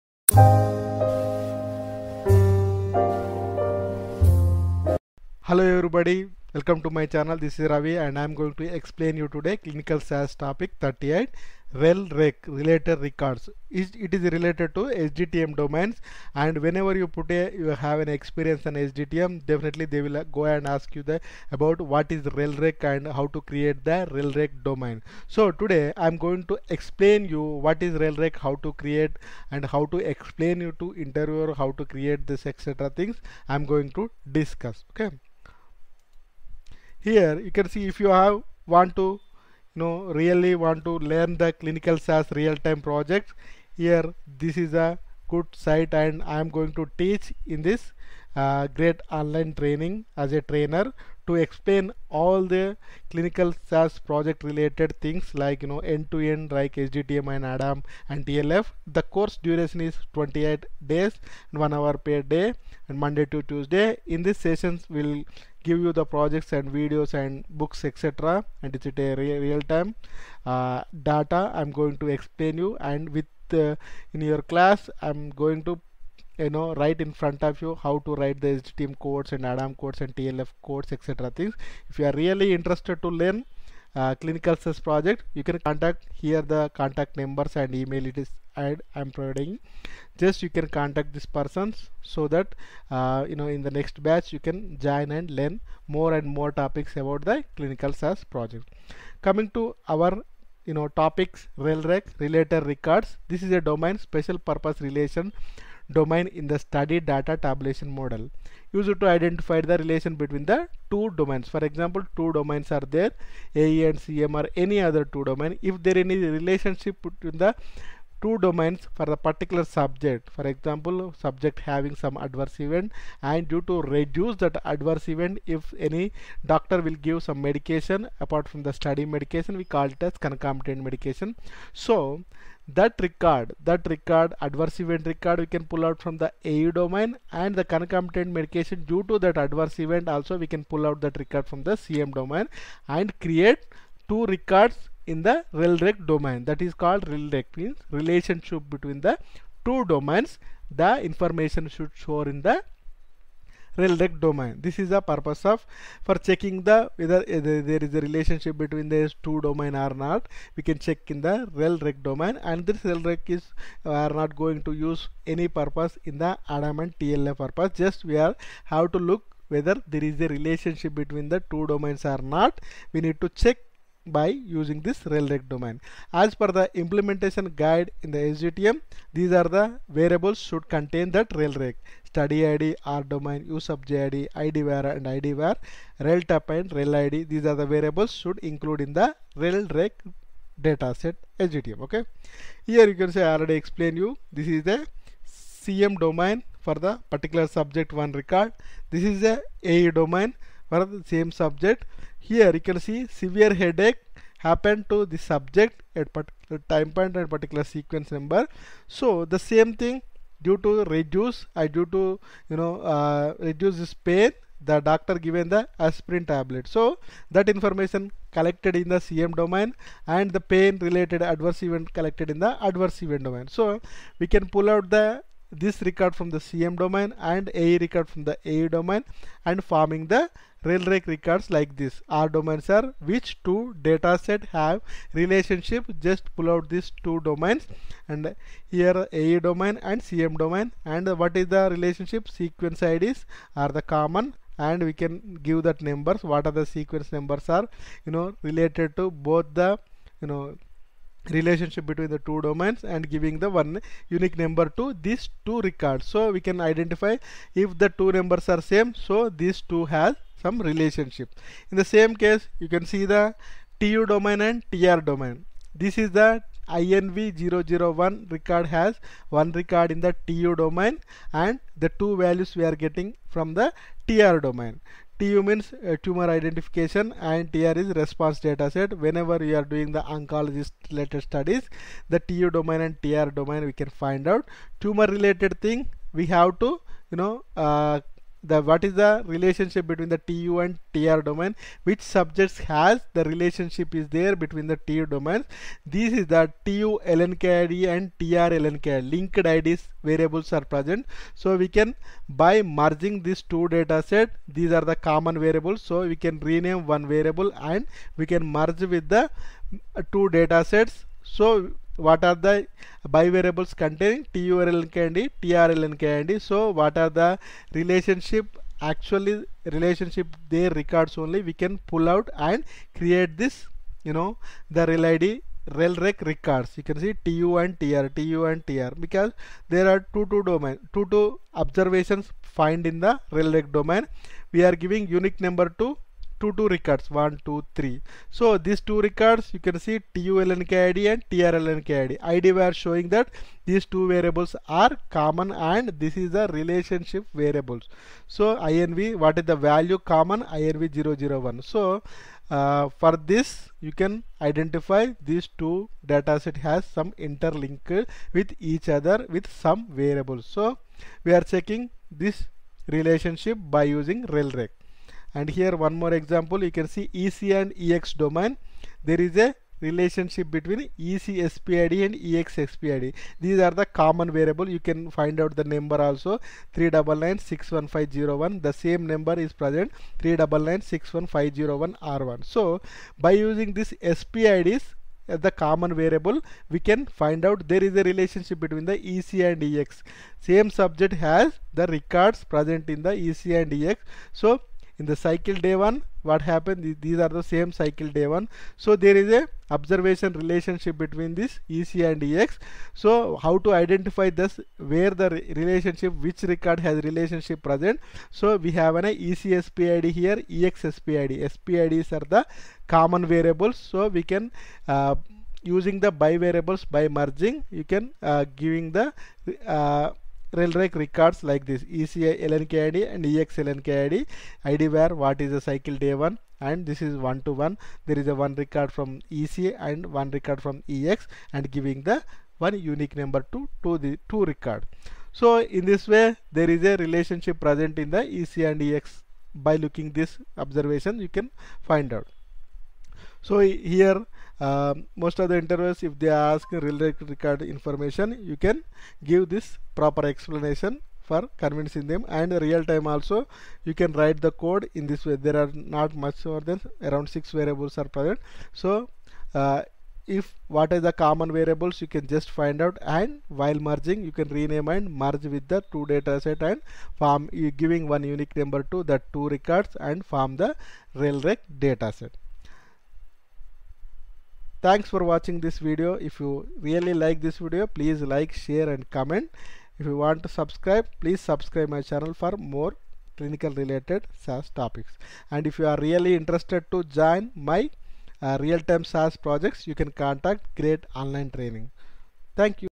Clinical SAS TOPIC 38 - RELREC
what is RELREC ?  Clinical interview topic #38 watch this video. For Real time clinical sas project training